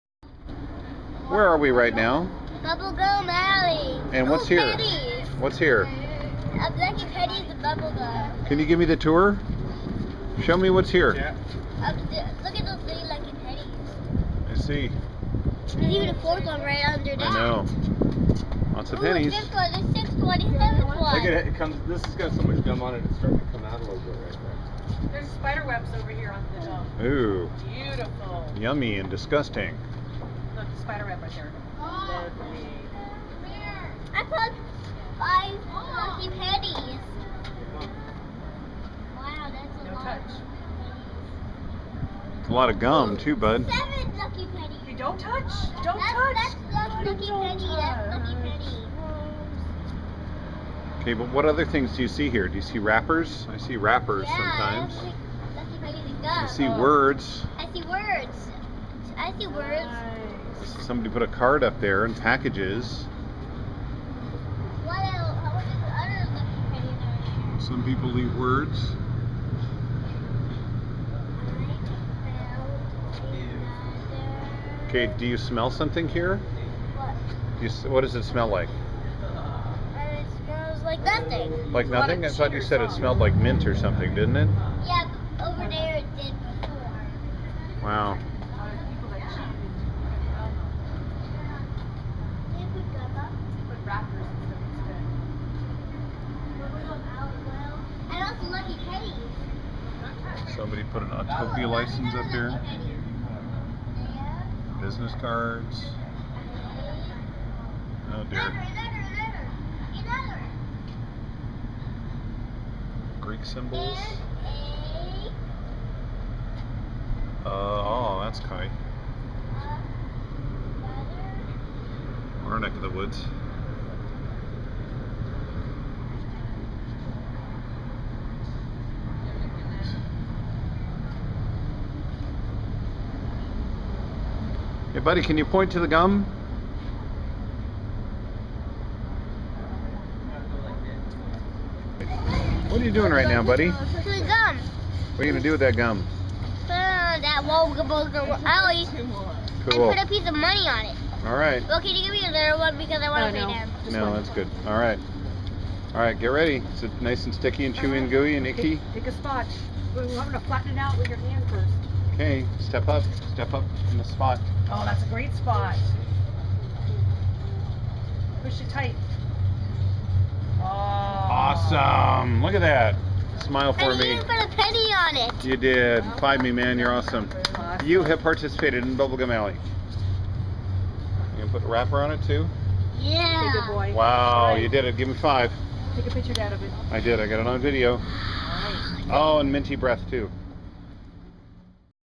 SLO's Bubble Gum Alley - BMan Leaves His Mark - July 2011
The BMan leaves his mark on 'Bubble Gum Alley,' the most lowbrow of tourist attractions in San Luis Obispo, California. People come for miles, just to leave already-been-chewed gum and other items stuck to the walls.

BMan borrowed his gum from total strangers, BTW. At least he asked first.